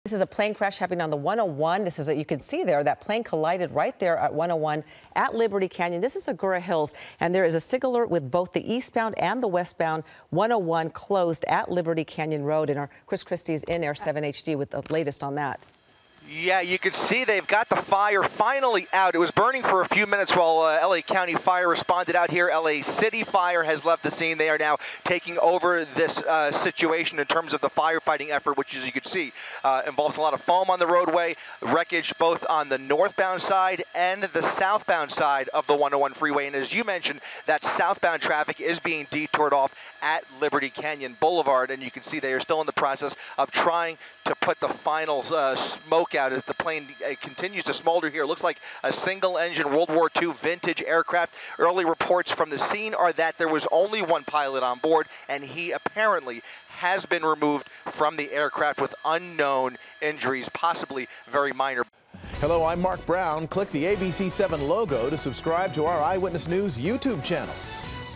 Small plane crashes on 101 Freeway in Agoura Hills | ABC7
A small plane crashed on the westbound 101 in Agoura Hills on Tuesday, and the fiery wreck shut down both sides of the freeway. Details